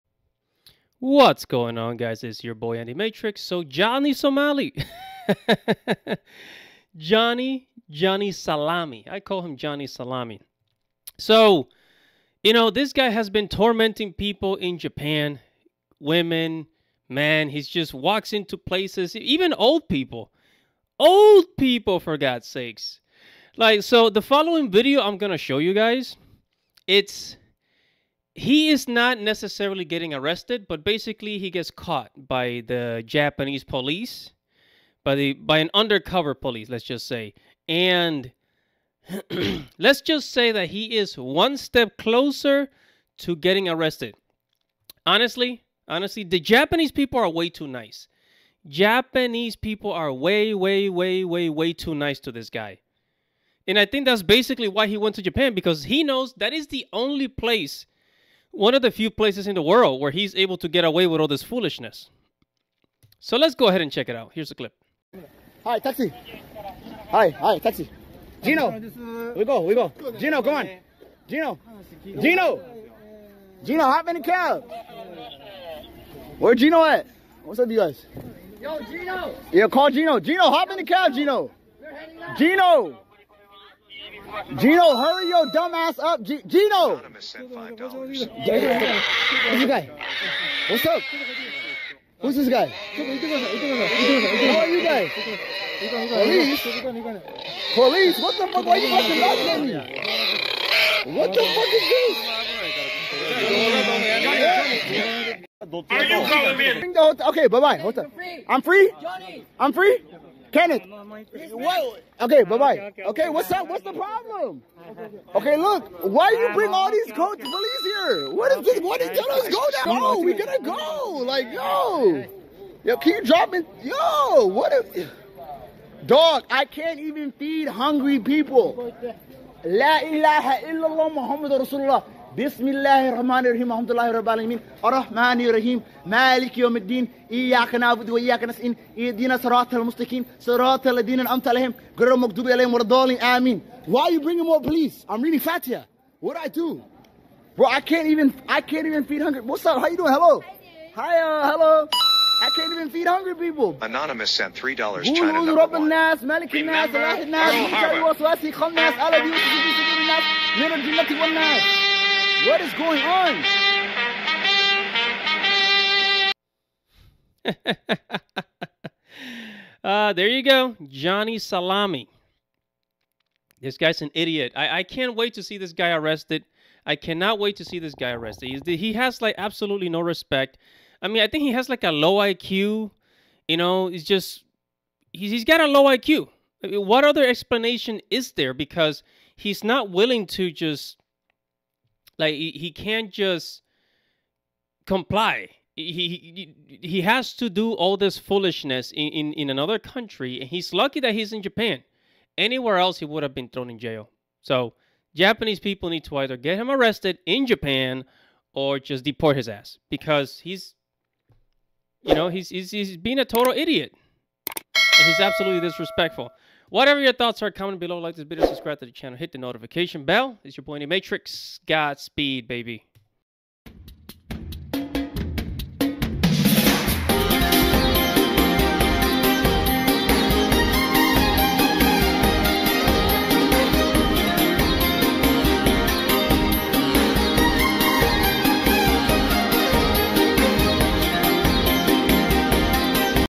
Johnny Somali Caught By Undercover Cop In Japan
Johnny Somali gets caught by an undercover cop. Unfortunately he was not arrested.